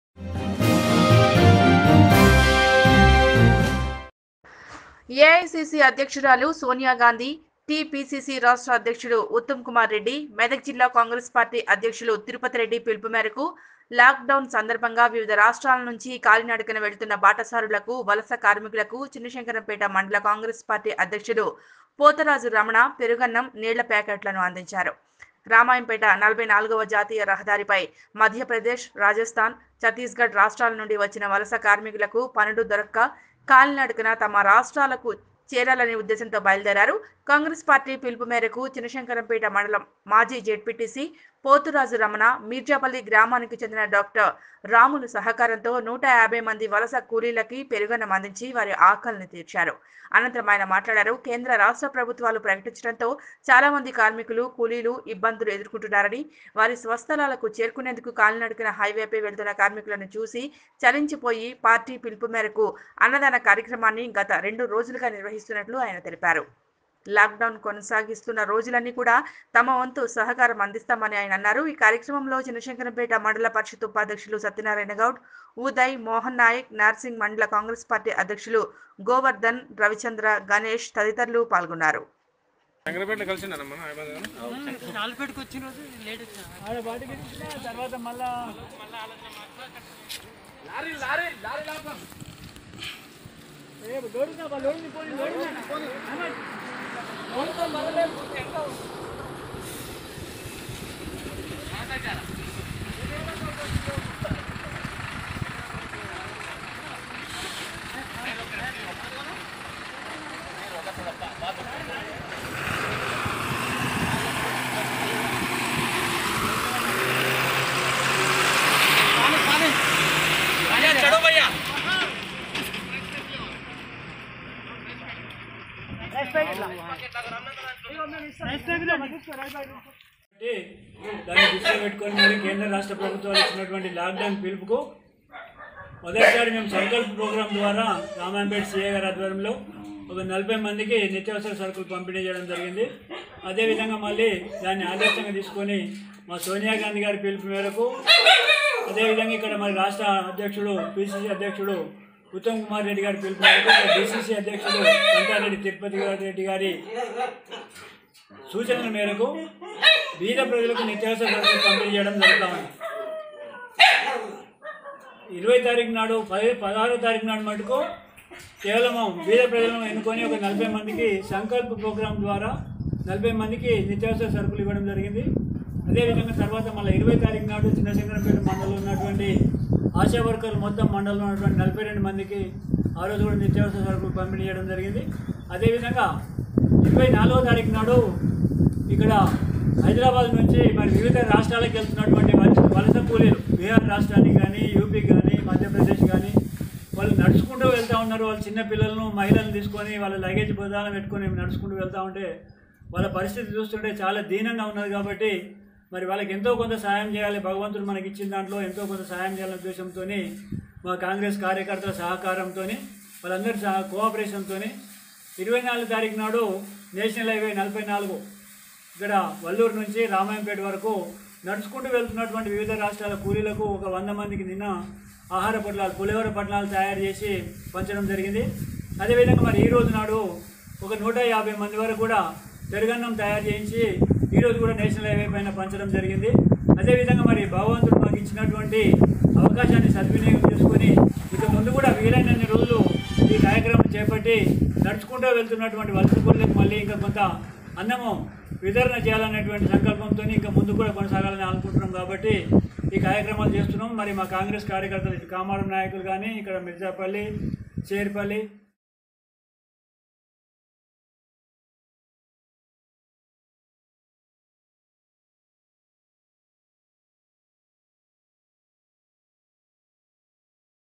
బాటసారులకు వలస కార్మికులకు కాంగ్రెస్ పార్టీ అధ్యక్షులు పోతరాజు రమణ పెరుగన్నం, నీళ్ల ప్యాకెట్లు
|| Nethra News || చిన్న శంకరం పేట ।।